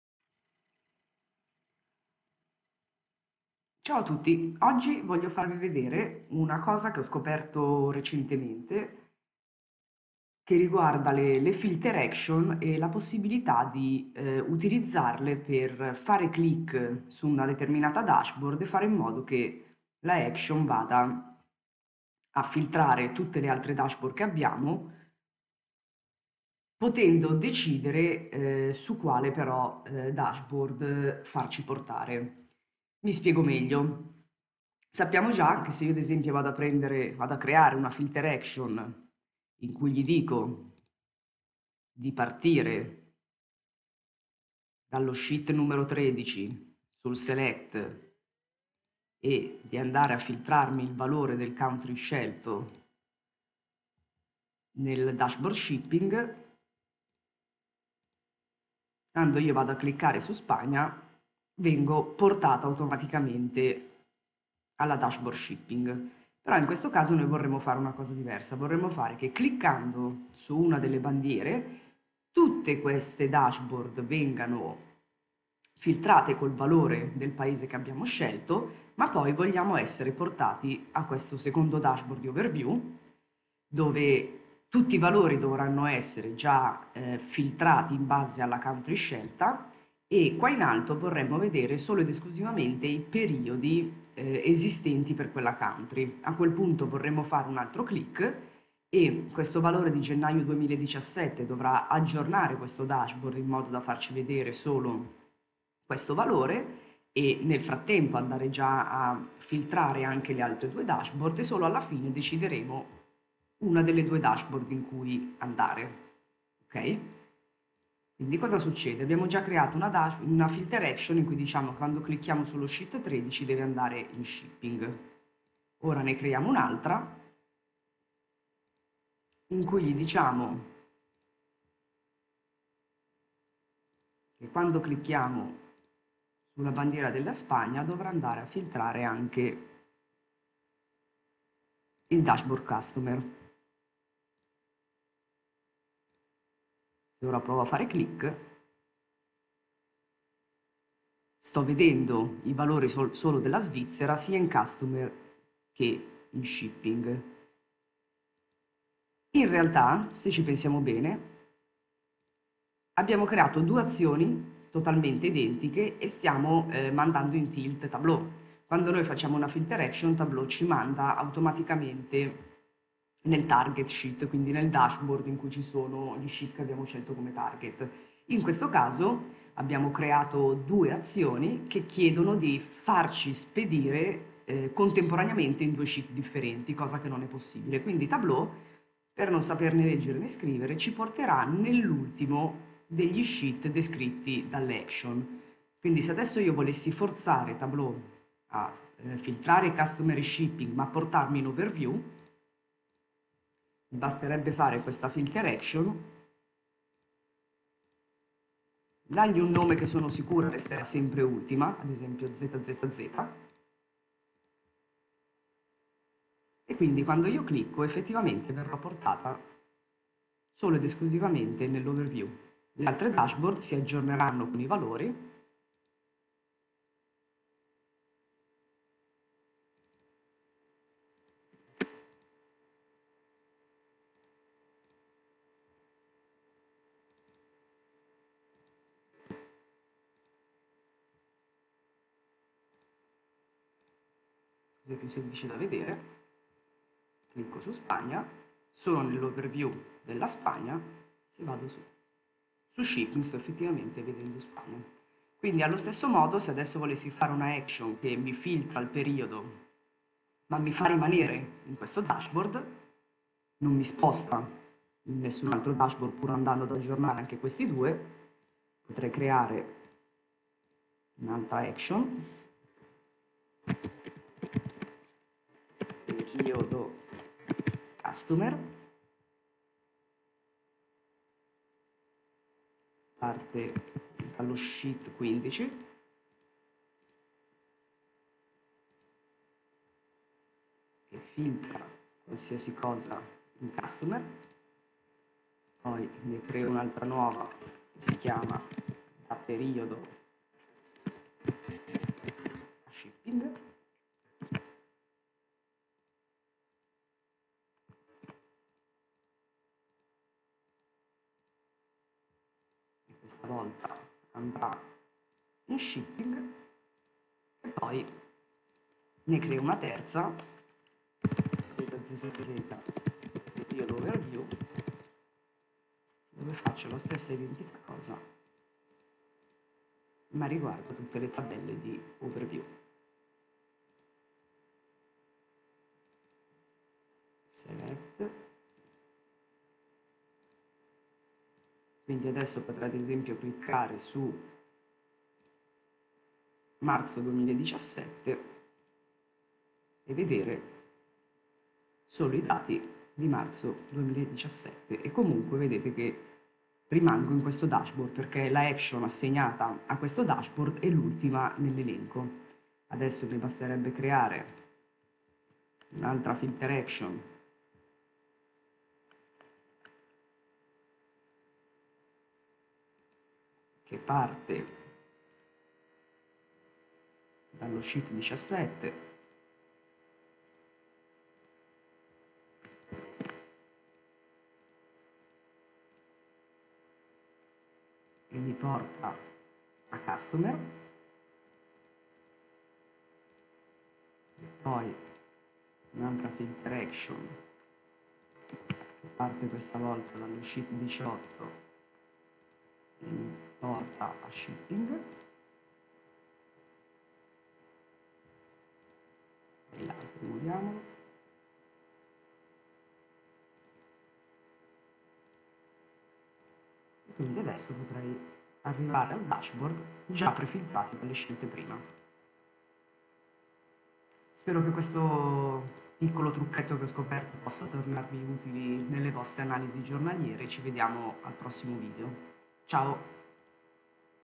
Filter action che si applicano a più dashboard
Vogliamo creare una filter action che al clic su un determinato mark vada a filtrare tante dashboard, magari anche tutte e allo stesso tempo ci porti un su un dashboard ben preciso.

Ad esempio abbiamo una dashboard che funge da homepage, in cui cliccando su una bandiera vogliamo filtrare i relativi dati nella prima dashboard di Overview, nella seconda con l'analisi dei clienti e anche nella terza con l'analisi delle spedizioni.

Filtra i dati in 3 dashboard differenti, e poi ci fa atterrare nel dashboard overview, dove avremo altre selezioni da fare.

Vediamo insieme come fare con questo breve video tutorial.